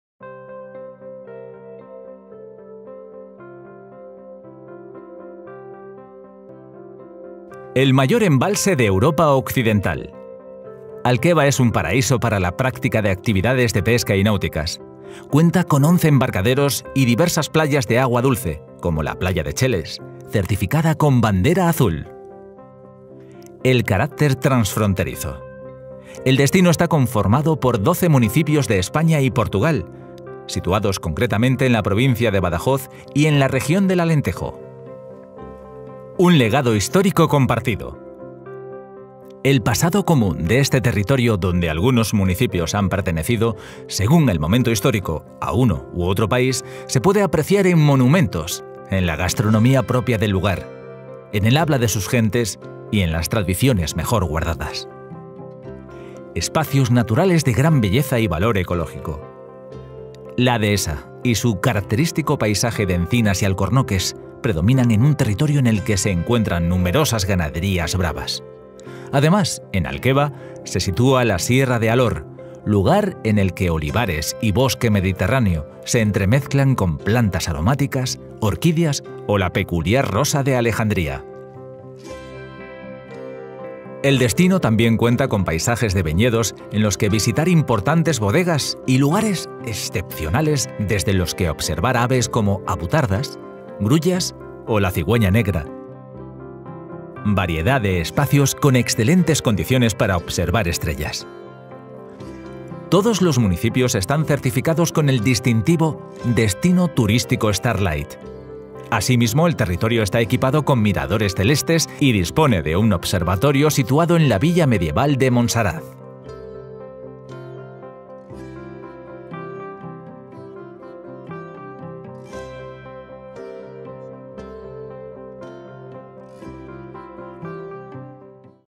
Alqueva
Uma divulgação feita por um organismo espanhol, mas que divulga também a parte portuguesa do Alqueva, o maior lago artificial da Europa - A disclosure made by a spanish organization, but that discloses also the portuguese part of Alqueva, the Europe's biggest artificial lake